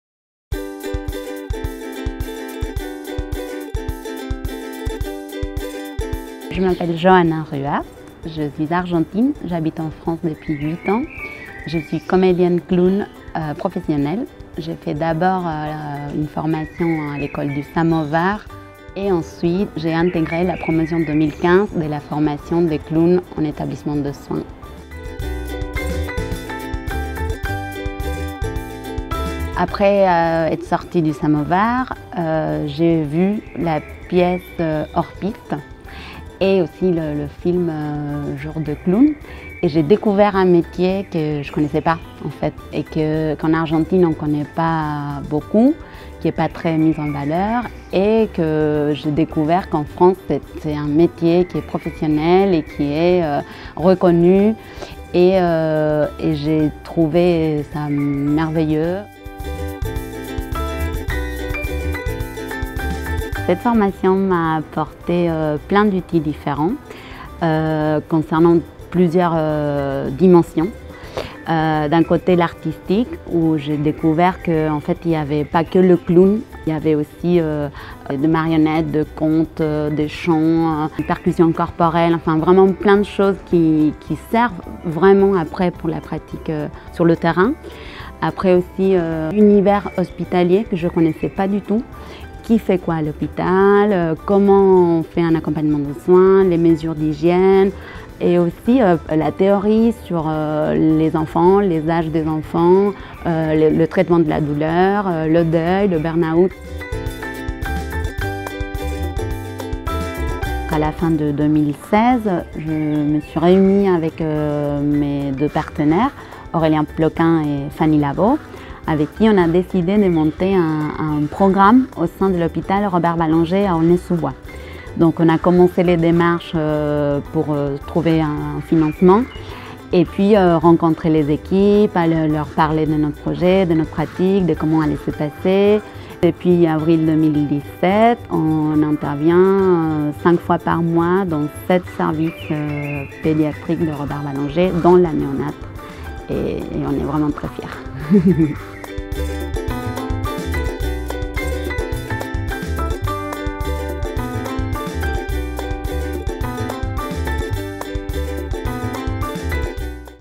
Johanna, ancienne stagiaire de la formation Comédien/ne-Clown en Etablissements de Soins CCES
Johanna raconte ce qui l'a motivé à suivre cette formation, ce qu'elle lui a apporté et son parcours depuis sa certification.